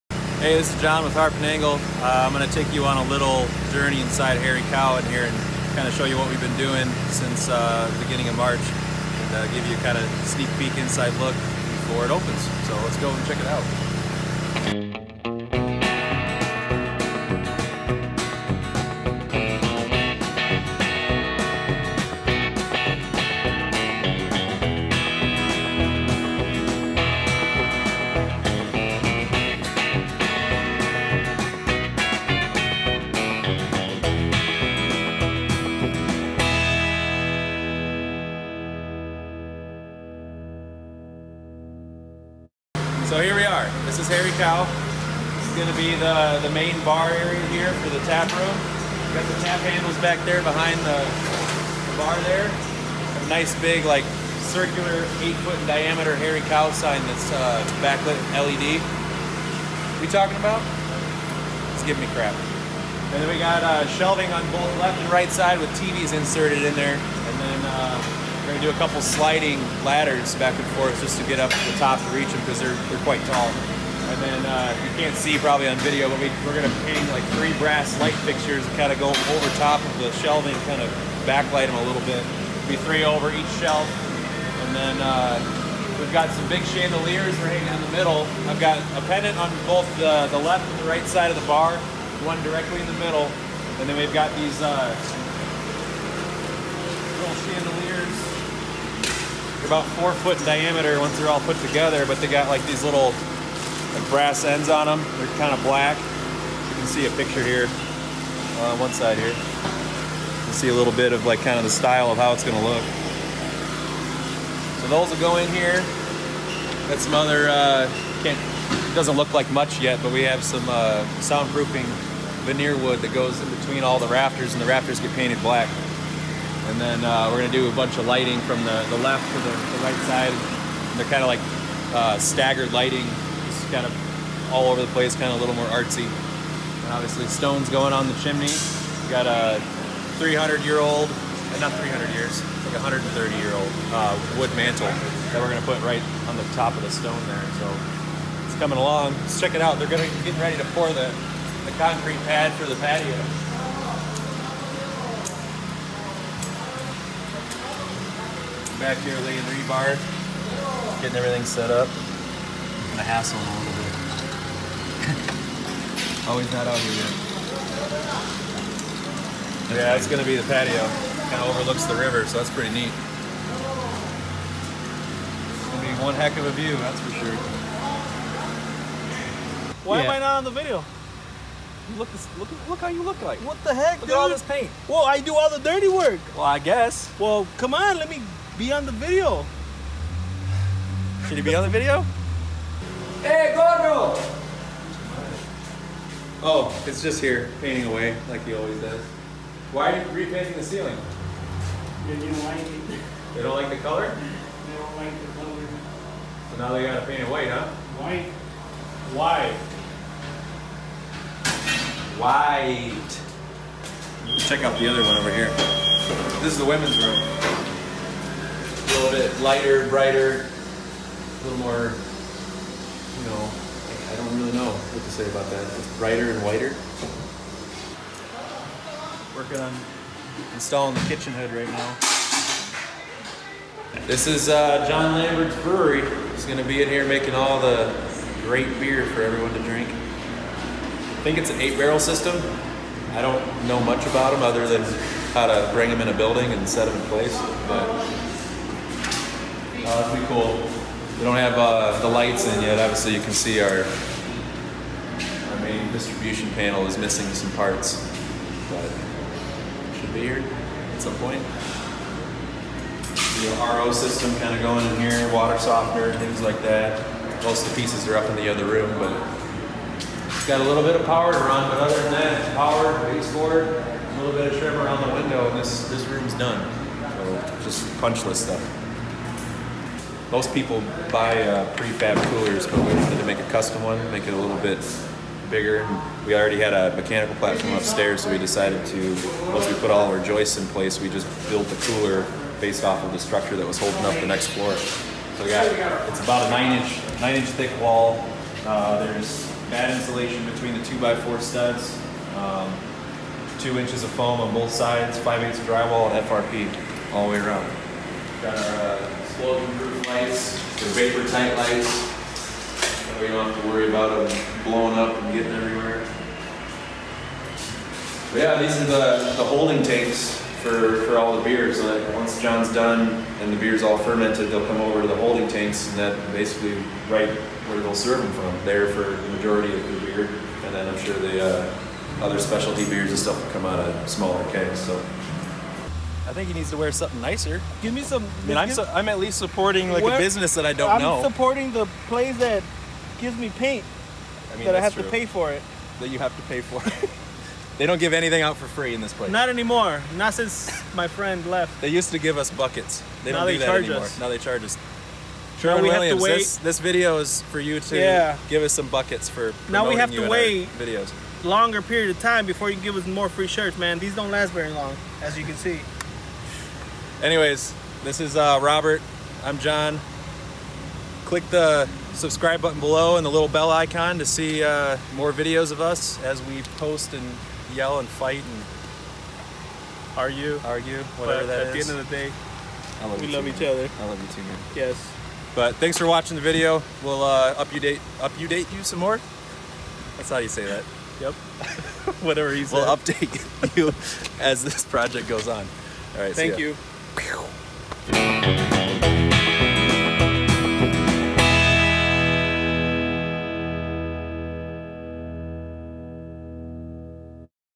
Building A Brewery
In this video we give an overview of where we are with the Hairy Cow Brewing Company.